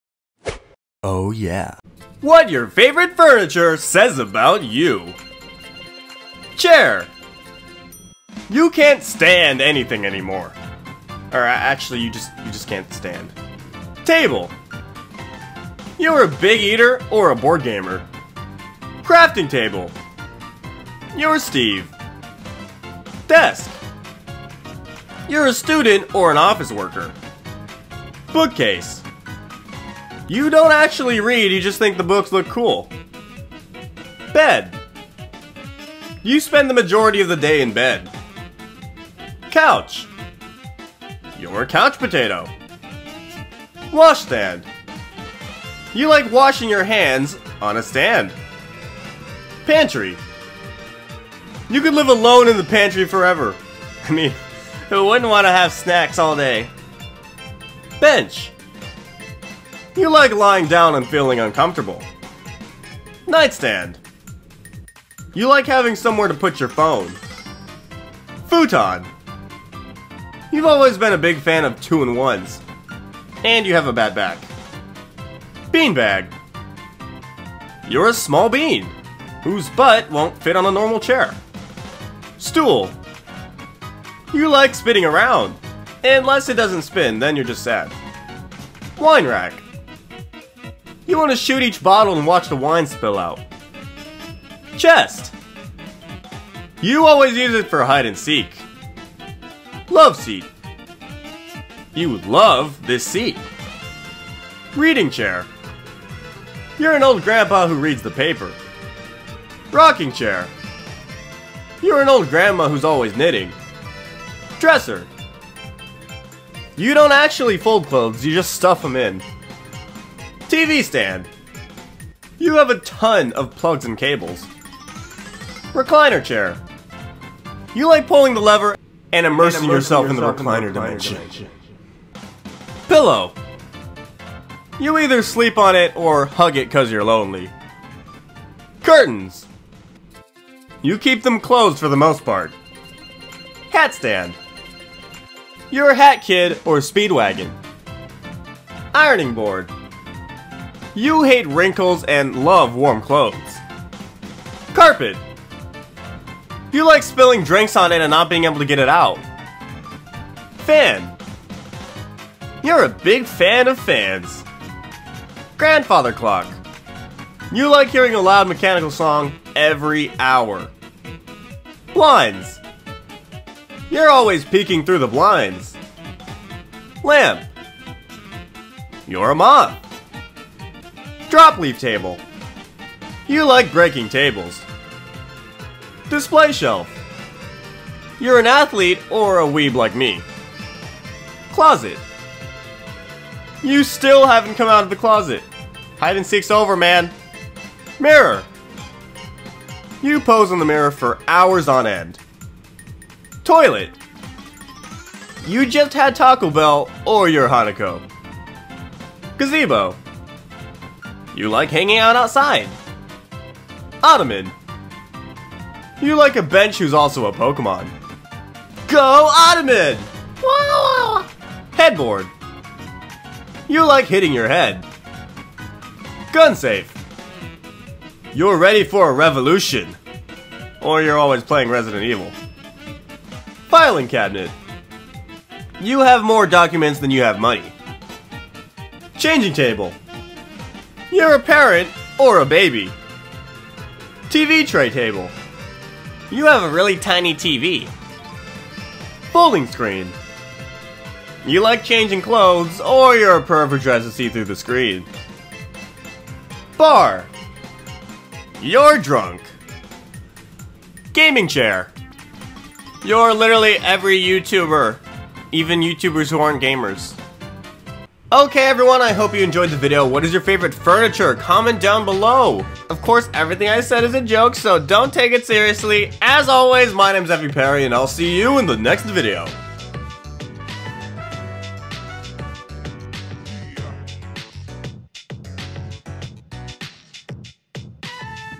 What your favorite furniture says about you!
Here is what your favorite furniture says about you! Ikea would be proud

►Copyright: If you are the creator of this song or the owner of